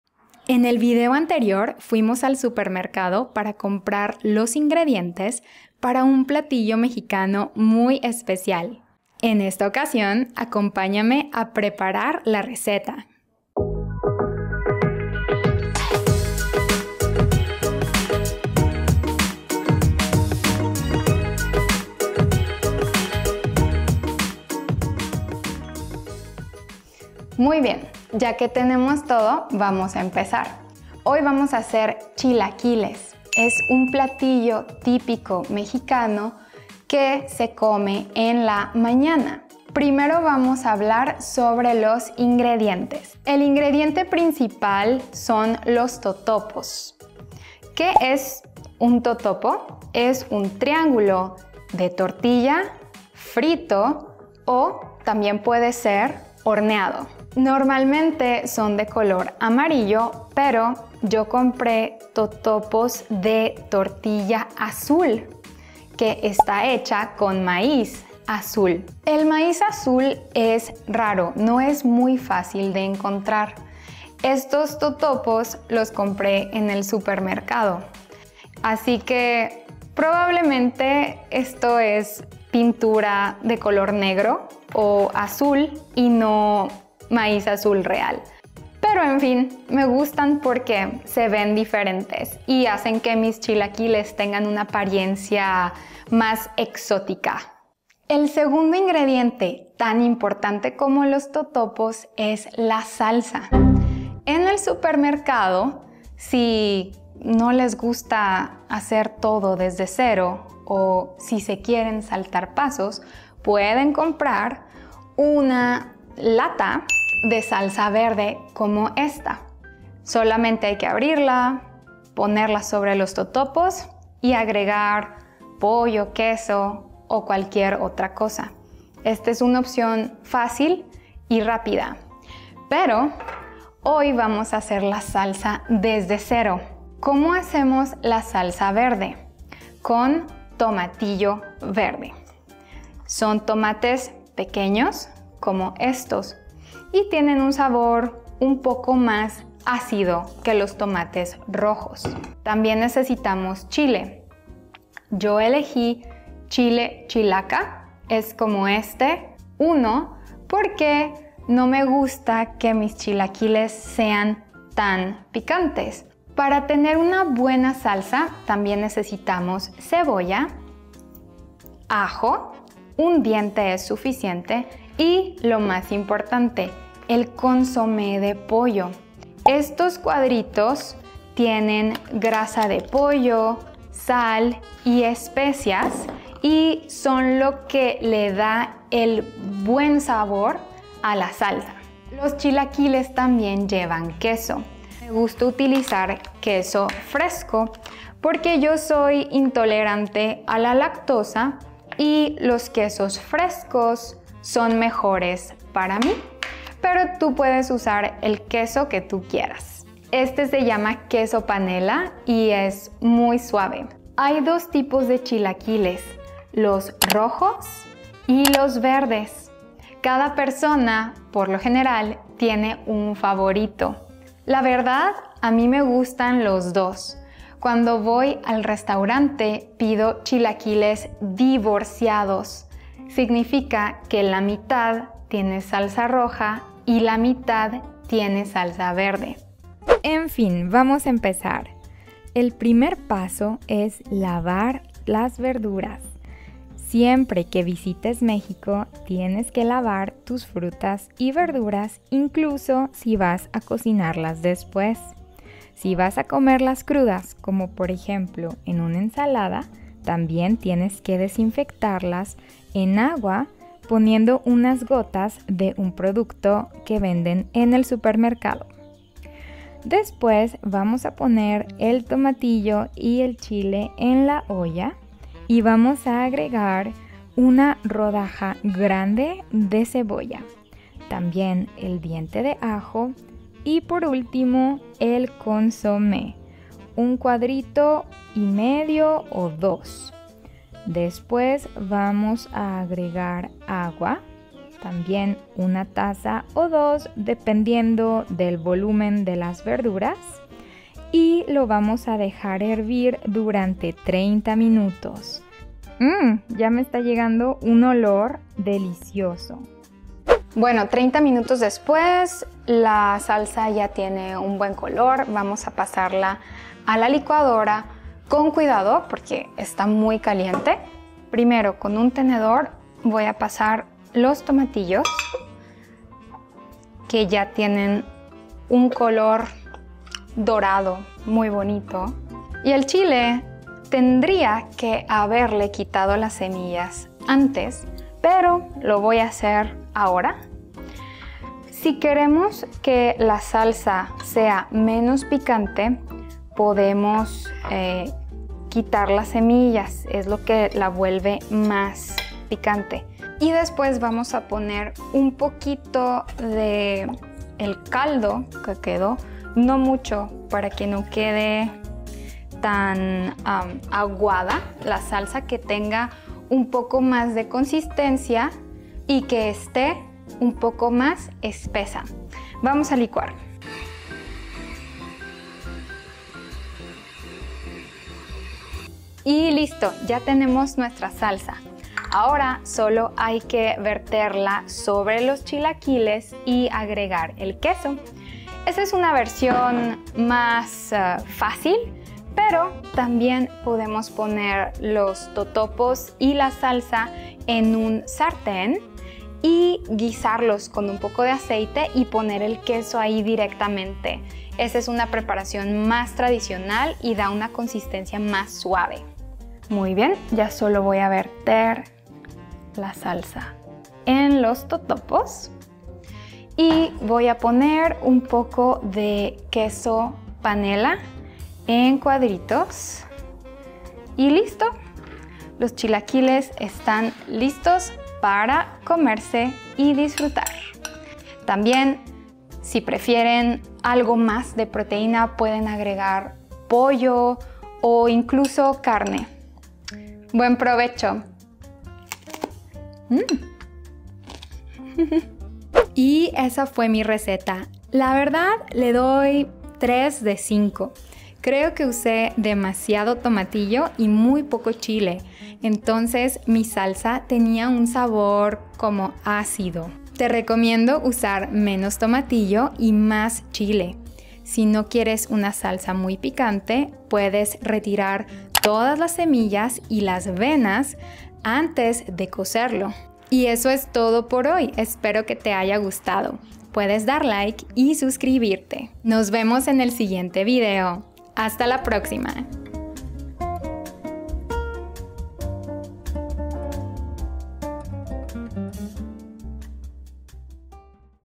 Making Chilaquiles | Learn SPANISH with this VIDEO BLOG
📖 Gael's Everyday Life storybook for beginners

00:00 Introducción
00:26 Los ingredientes
04:00 La preparación
06:50 A comer